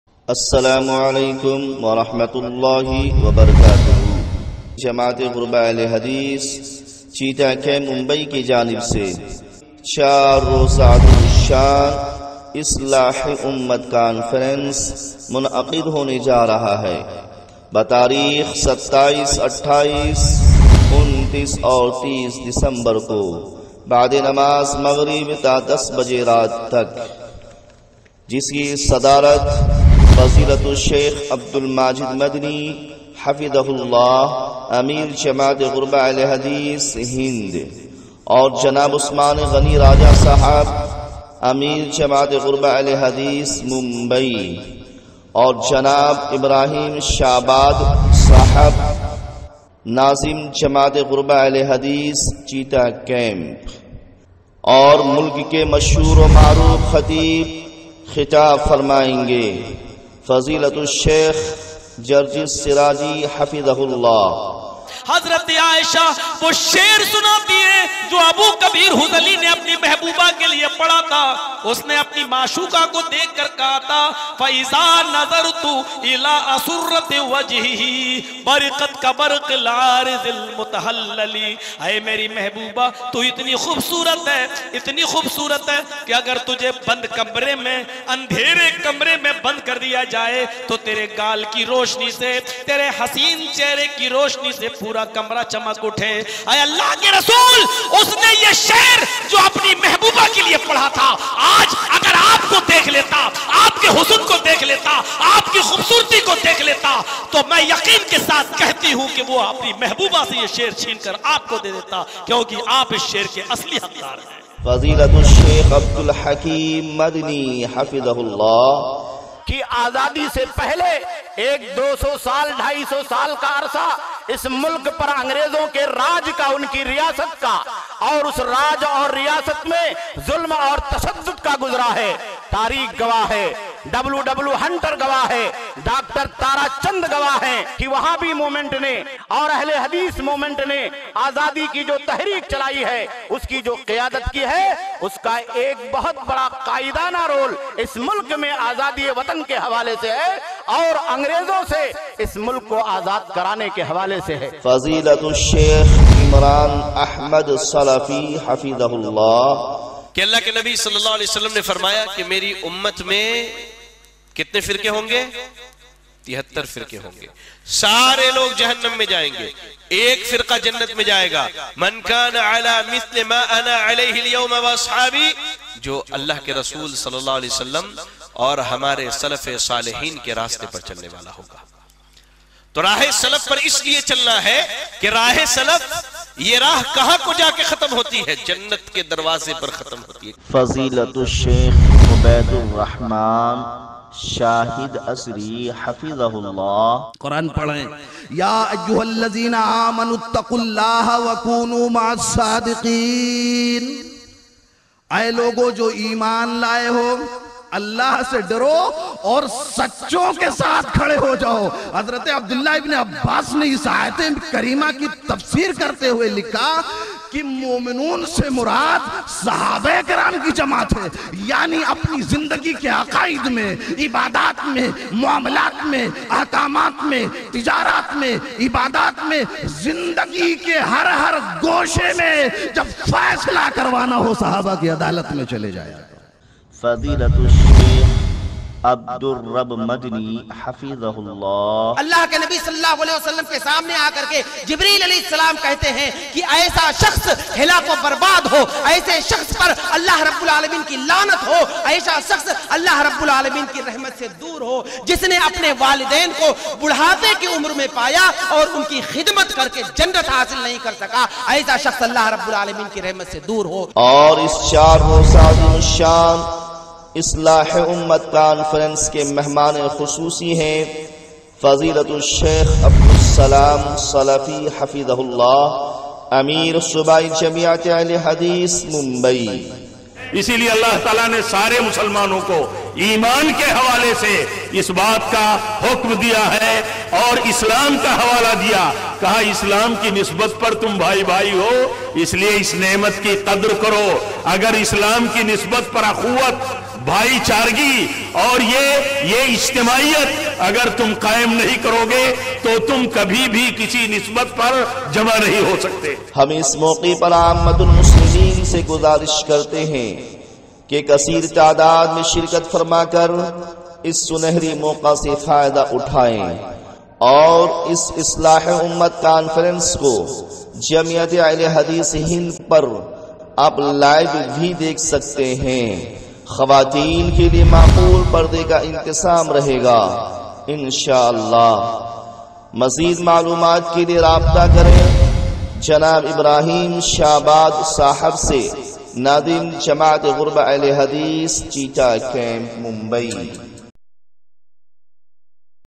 Promo | 4 Roza Azeem O Shaan Islah E Ummat Conference | 27 to 30 Dec 2019 | 6PM | Cheeta Camp Mumbai
Madrasa Muhammadia Wa Masjid Jamaat E Ghuraba Ahle Hadees Cheeta Camp Mumbai Presents

4 Roza Azeem O Shaan Islah E Ummat Conference

 Insha Allah
Date: 27, 28, 29 & 30 December 2019 (Friday to Monday)

Timing: After Maghrib To 10:00 PM

Venue: Near Janta Hotel 27,28 Dec 2019 &  Near Dosti Hotel 29,30 Dec 2019 Cheeta Camp, Mumbai, 400088.

Zere Imarat:
Shaikh Abdul Majid Madani (Ameer Jamaat E Ghuraba Ahle Hadees Hind)

Zere Sadarat:
Janab Usman Ghani Raja Sahab (Ameer Jamaat E Ghuraba Ahle Hadees Mumbai)

Under The Supervision:
Janab Ibrahim Shabad Sahab  ( Nazim Jamaat E Ghuraba Ahle Hadees Cheeta Camp)

Speakers:
Shaikh Jarjees Siraji
Shaikh Abdul Hakeem Madani
Shaikh Imran Ahmed Salafi
Shaikh Ubaid Ur Rahman Shahid Asari
Shaikh Abdur Rab Madani

Mehman E Kususi:
Shaikh Abdul Salam Salafi (Ameer Subai Jamiat Ahle Hadees Mumbai)

Note: Special Audiovisual Arrangement For Ladies.

Organized by: Madrasa Muhammadia Wa Masjid Jamaat E Ghuraba Ahle Hadees Cheeta Camp